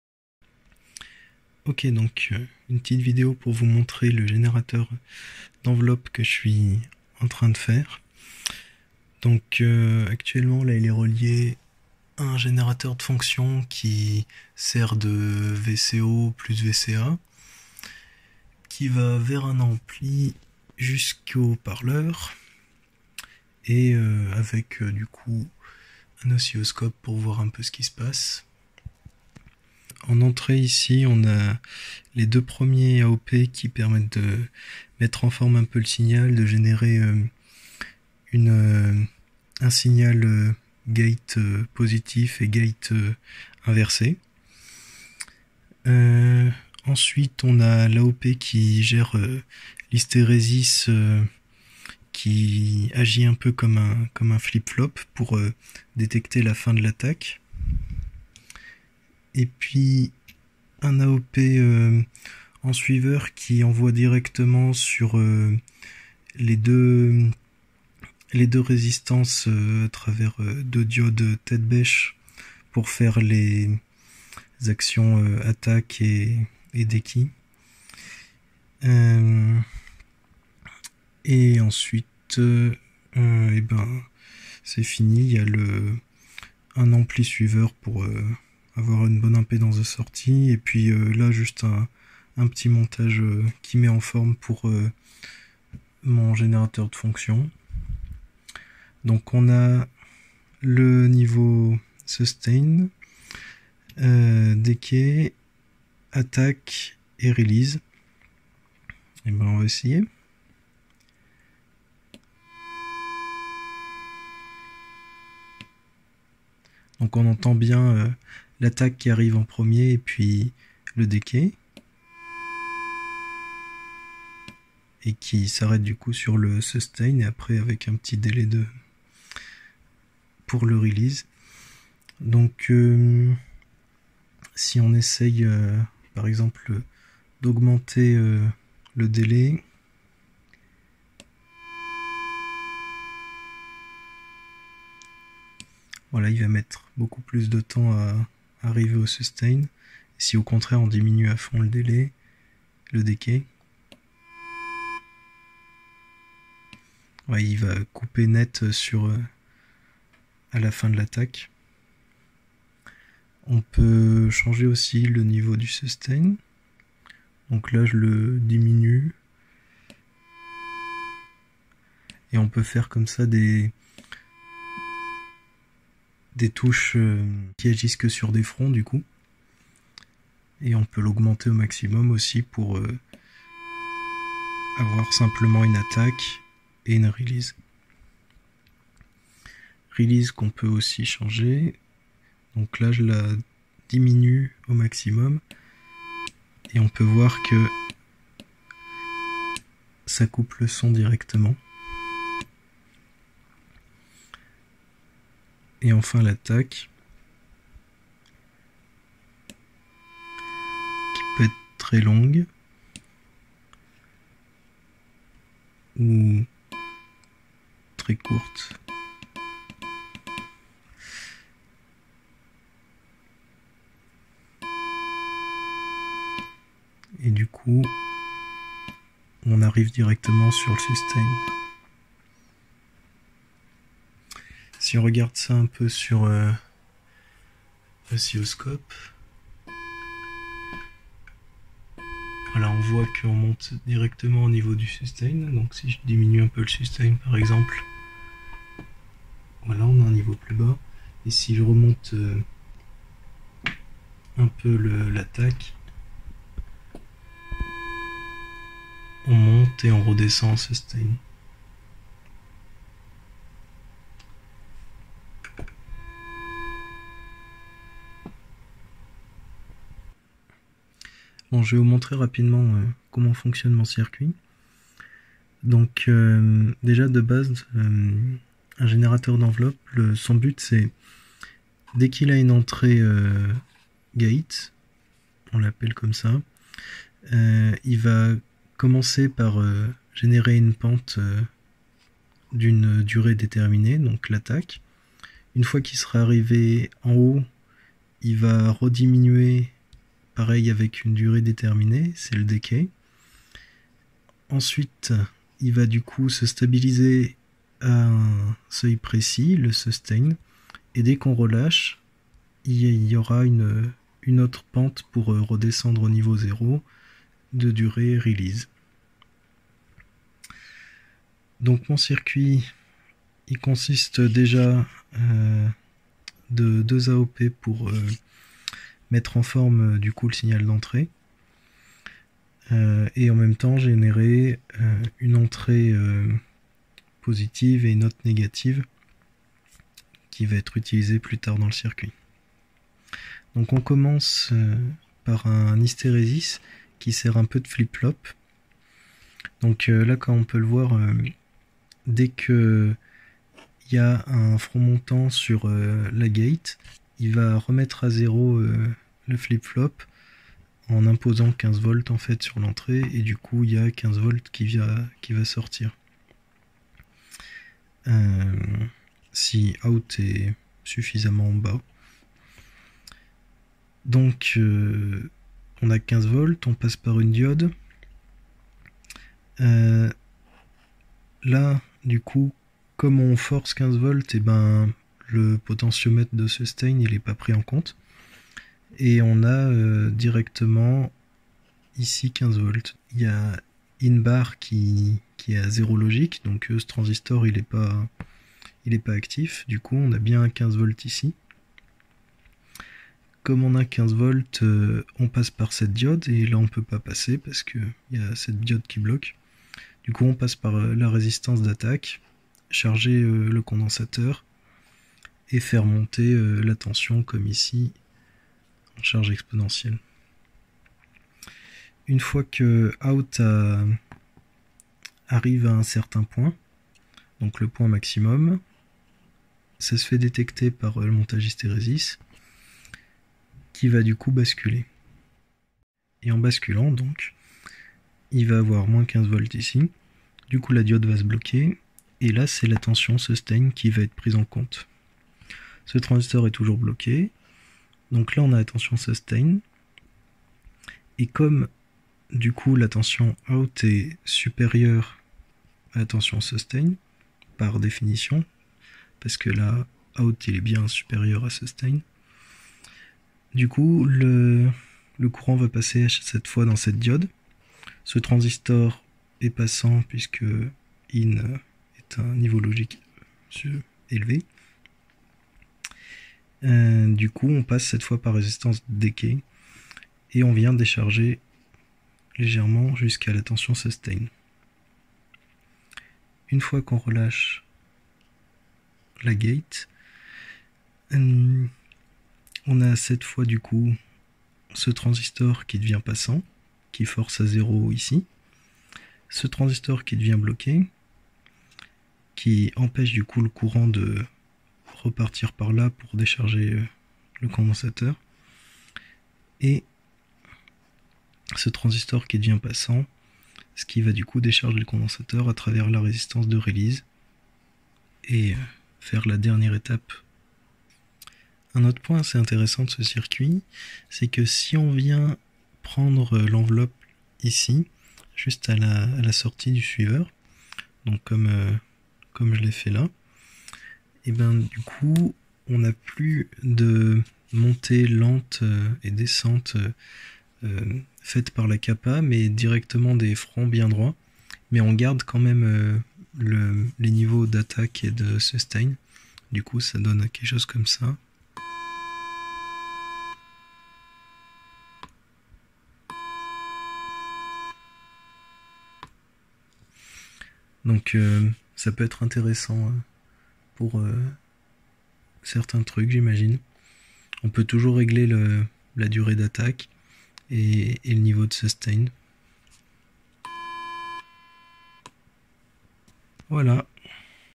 Full ADSR envelope generator with only opamp (LM741)
In this video (with English subtitles), I will show you this simple circuit, and also explain it a bit.
I managed to make a complete envelope generator using only opamp and a couple of side components. The only downside is that you can't use CV on it, and it's not really tweakable, but eh, it works!

0:00 Setup
0:30 Rough explanation
1:50 Demo
4:00 Scope view
4:30 ASDR explanation
5:20 Schematic explanation
8:40 Bonus